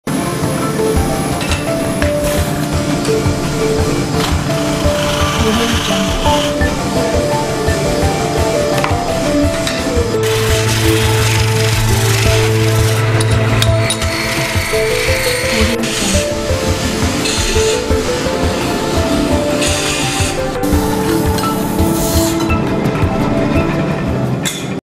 Coppersmith Working - Stock Video
Coppersmith Working

A coppersmith, also known as a redsmith, is a person who makes artifacts from copper. The term redsmith comes from the colour of copper. Copper is generally considered to be a soft metal, meaning it can be worked without heating. Over a period of working the metal in this way it can ‘work harden’.

4 Shots (FULL-HD 1920×1080)